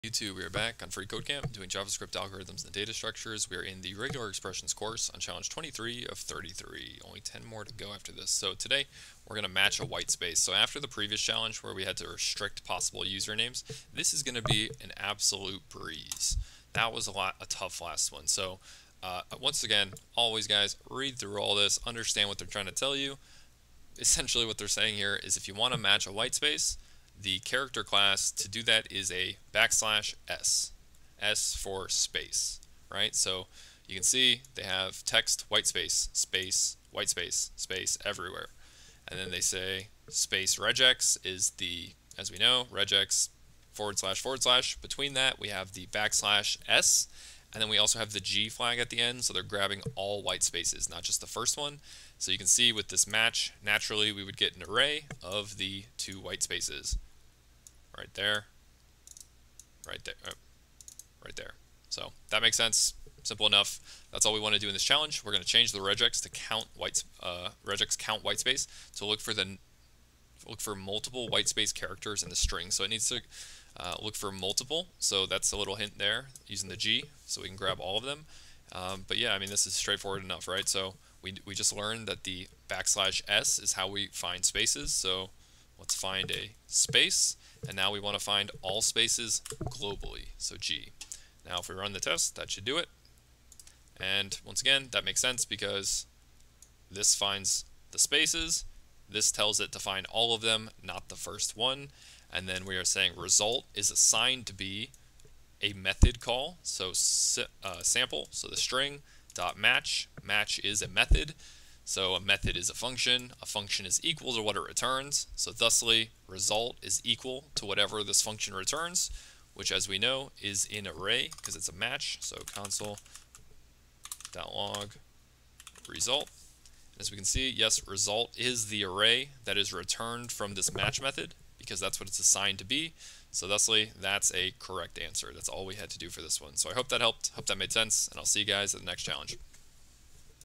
23 Match Whitespace - RegEx - FreeCodeCamp EXPLAINED - JavaScript Algorithms Data Structures - FCC
To assist you if you get stuck, and break down core concepts the material is trying to convey.

FreeCodeCamp - Javascript Algorithms & Data Structures Course
Regular Expressions Sub-course
Challenge 23 Match Whitespace